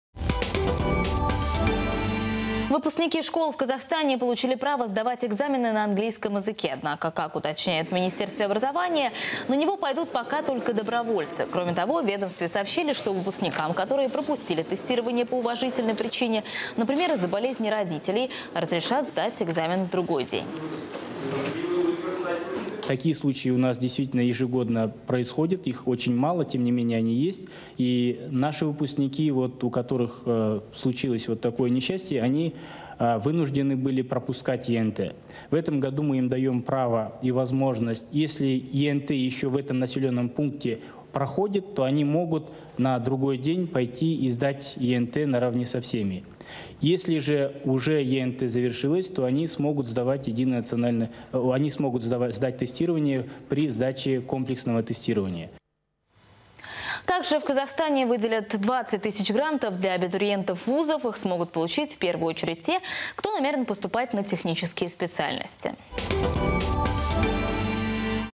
Выпускники школ Казахстана смогут сдавать ЕНТ на английском языке
Выпускники школ в Казахстане получили право сдавать экзамены на английском языке, передает «МИР 24».